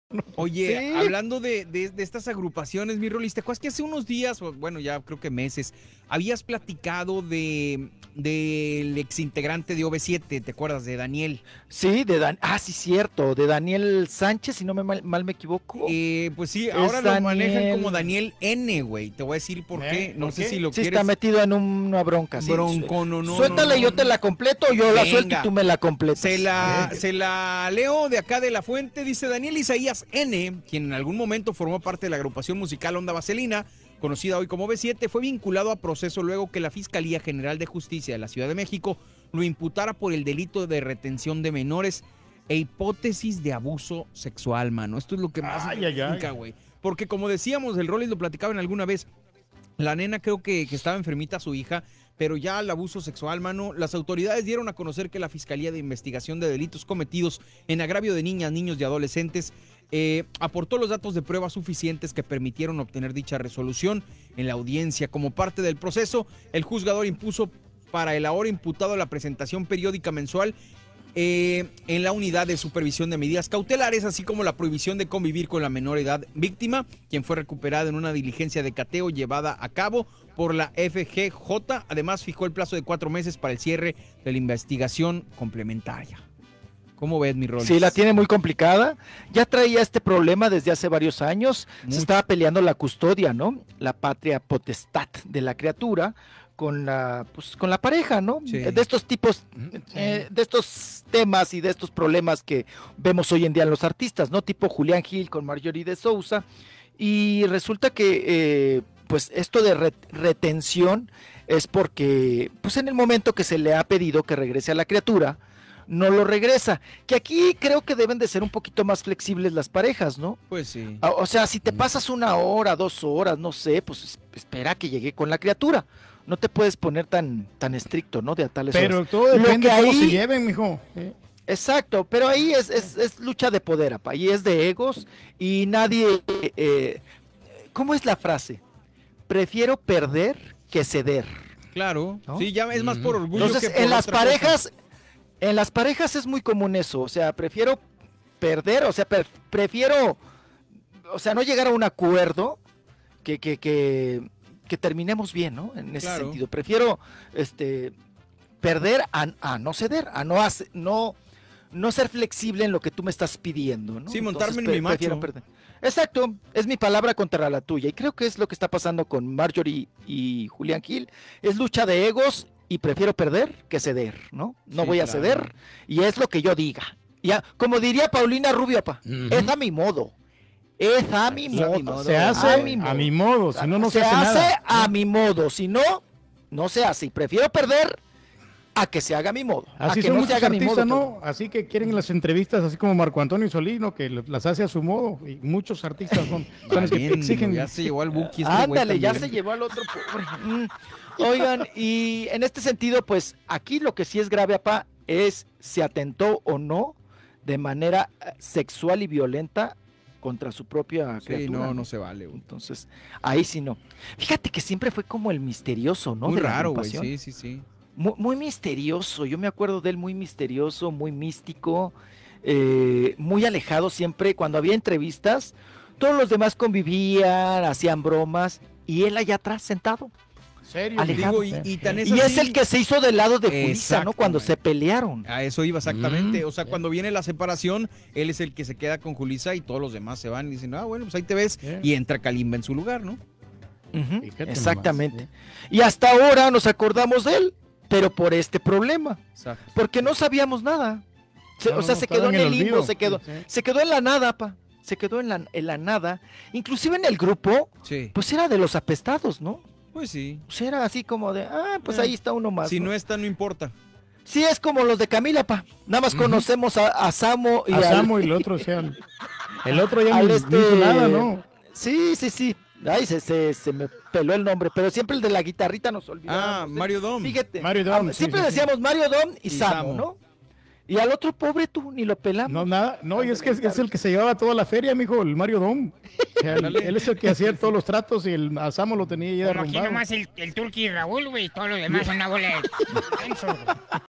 DANIEL EX – INTEGRANTE DE ONDA VASELINA IMPLICADO EN UN DELITO
El ex integrante de la Onda Vaselina ahora podría enfrentar a la justicia debido a una supuesta sustracción y retención de menores. Esta acusación la hace su pareja debido a que el cantante no regresa a su hija a tiempo y podrían existir ciertos abusos que se están investigando.